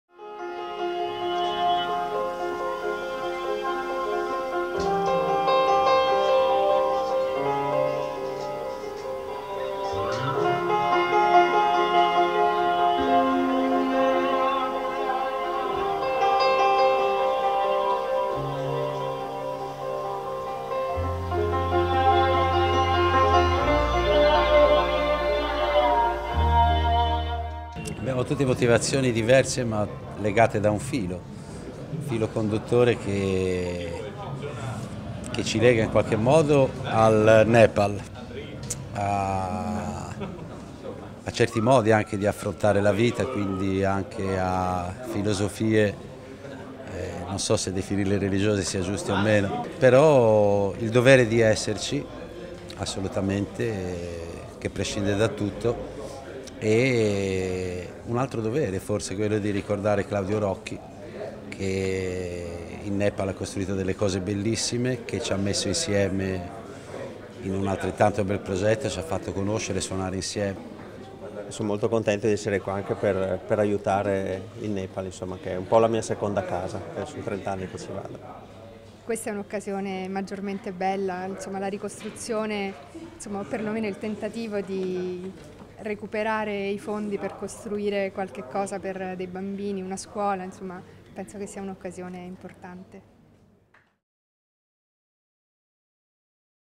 ASIA - Gianni Maroccolo, Alessandra Celletti e Beppe Brotto per il Nepal
Gianni Maroccolo, Alessandra Celletti e Beppe Brotto hanno aderito al progetto di ASIA per la costruzione di una scuola in Nepal. Partecipa